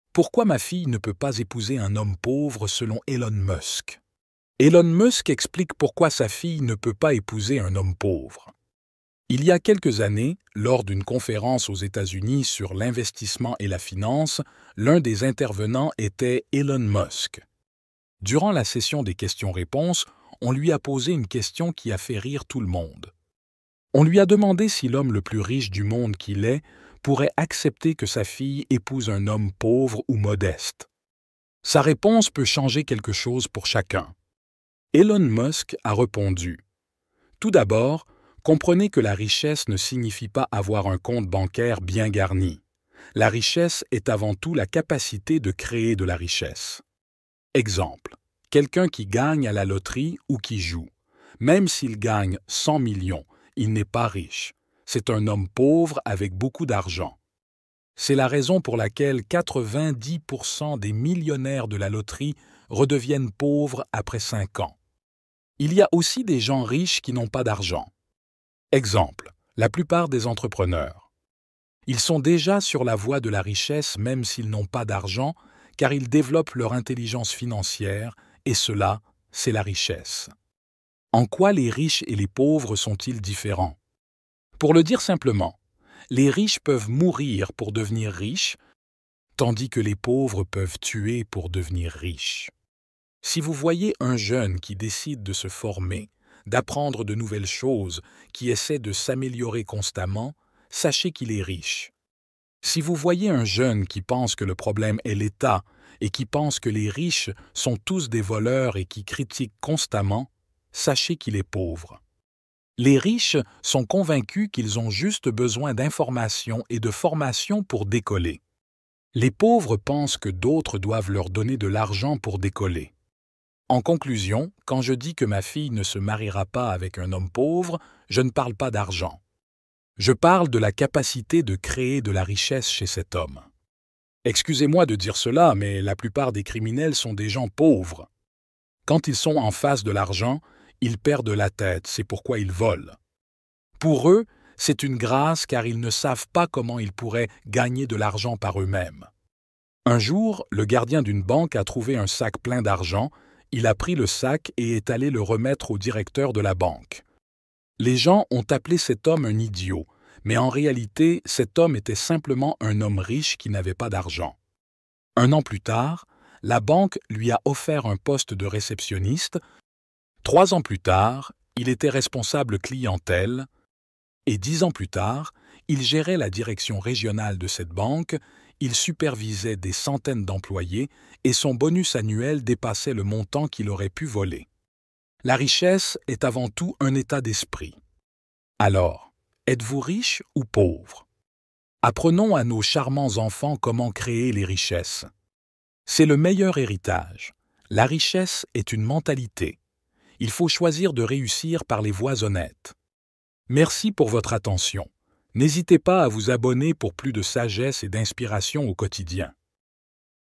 Pourquoi ma fille ne peut pas épouser un homme pauvre selon Elon Musk.
Elon Musk a expliqué lors d'une conférence l'importance de la richesse et la raison pour laquelle il ne souhaite pas que sa fille épouse un homme pauvre. Pour lui, la richesse ne se résume pas à un compte en banque bien garni, mais plutôt à la capacité de créer de la richesse. Il donne l'exemple des gagnants de loterie qui, malgré leur fortune soudaine, finissent souvent par retomber dans la pauvreté car ils n'ont pas acquis les compétences nécessaires pour gérer leur argent.

Il souligne également que la richesse ne se limite pas à l'argent, mais peut également être la capacité à développer son intelligence financière. Selon lui, les entrepreneurs sont déjà riches, même s'ils n'ont pas forcément beaucoup d'argent, car ils sont sur la voie de la richesse en développant leurs compétences et leurs connaissances.

Il explique que la différence entre les riches et les pauvres réside dans leur mentalité. Les riches sont prêts à travailler dur pour atteindre leurs objectifs, tandis que les pauvres cherchent souvent des excuses ou attendent que l'argent leur soit donné. Il affirme également que la plupart des criminels sont des personnes pauvres, car elles ne savent pas comment gagner de l'argent légalement.

En conclusion, Elon Musk indique que lorsqu'il parle de ne pas vouloir que sa fille épouse un homme pauvre, il ne parle pas seulement d'argent, mais de la capacité à créer de la richesse. Il considère que la richesse est avant tout un état d'esprit et encourage les parents à enseigner à leurs enfants comment créer leur propre richesse de manière honnête.